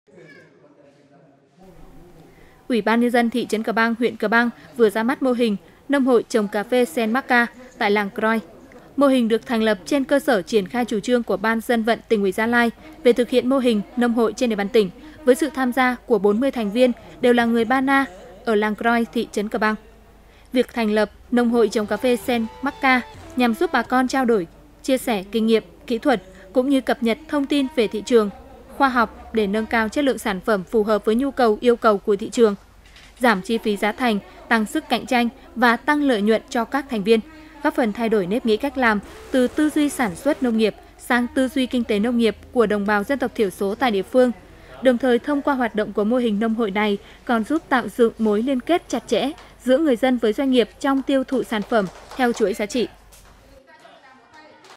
Thị trấn Kbang ra mắt “Nông hội trồng cà phê xen mắc ca” làng Groi
UBND thị trấn Kbang, huyện Kbang vừa ra mắt mô hình “Nông hội trồng cà phê xen mắc ca” tại làng Groi. Mô hình được thành lập trên cơ sở triển khai chủ trương của Ban Dân vận Tỉnh ủy Gia Lai về thực hiện mô hình “Nông hội” trên địa bàn tỉnh với sự tham gia của 40 thành viên đều là người Bahnar ở làng Groi (thị trấn Kbang).